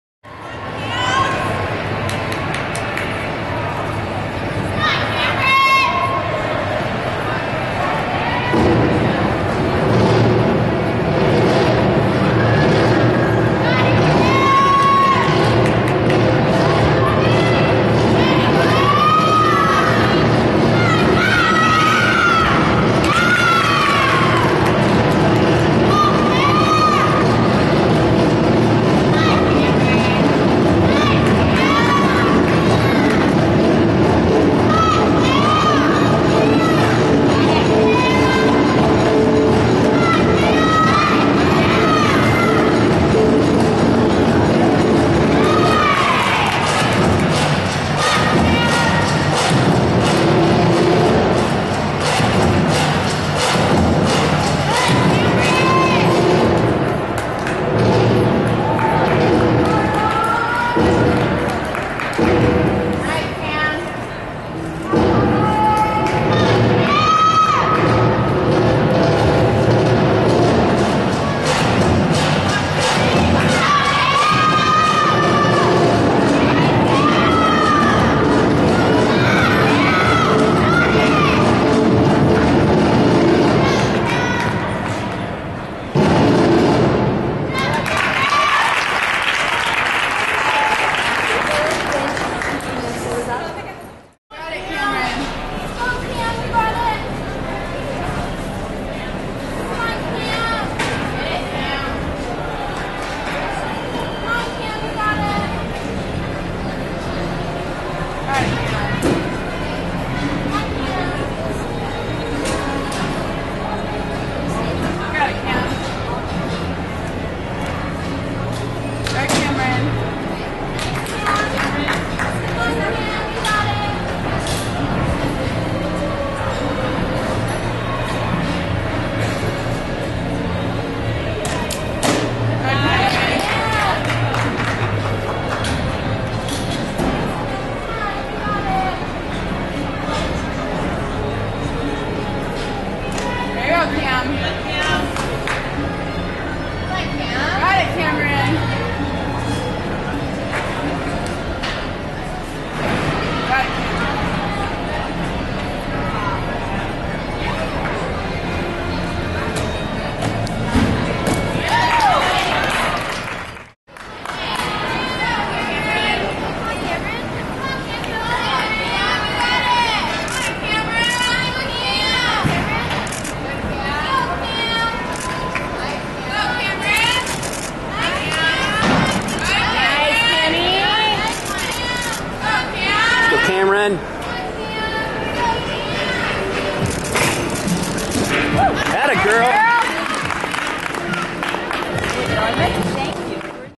Kamryn Bayer - San Antonio Classic 1/14
Floor, Beam, and Bar routines from Alamo Classic Meet in San Antonio
Texas - 1/14